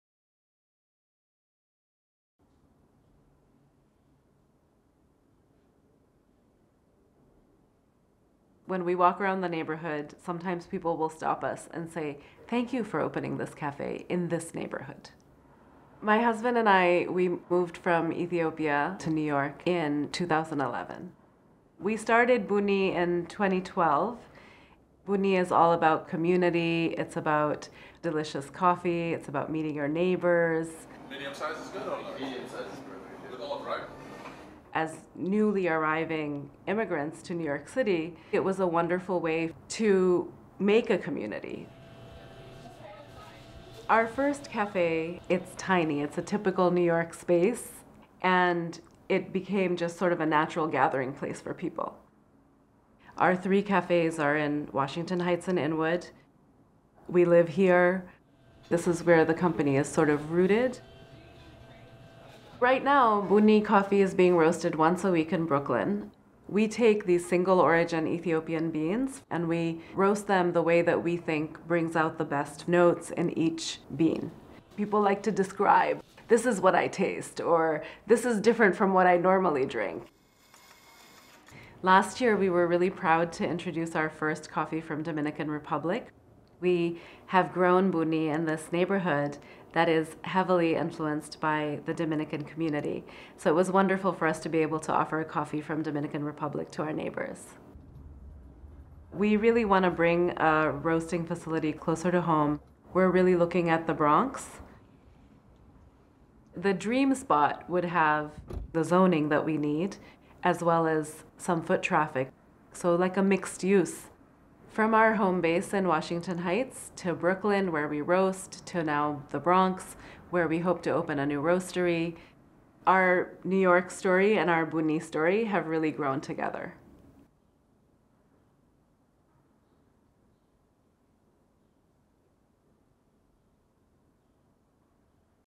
Meet Sarina Prabasi, Accompany Capital 2023 Immigrant Heritage Week Made in New York Award Winner
Meet Sarina Prabasi, co-founder and co-owner of Buunni Coffee and this year's Accompany Capital Immigrant Heritage Week Made in New York Award winner.
Sarina, who is originally from Nepal, and her husband Elias, who is Ethiopian, take pride in creating community & culture around delicious Ethiopian coffee in their welcoming uptown spaces, now with three locations in Upper Manhattan!

Learn more about Buunni Coffee and it's Washington Heights and Inwood locations

Video directed by Maria Luisa Gambale and Camille June Gregoire, Vera Donna Media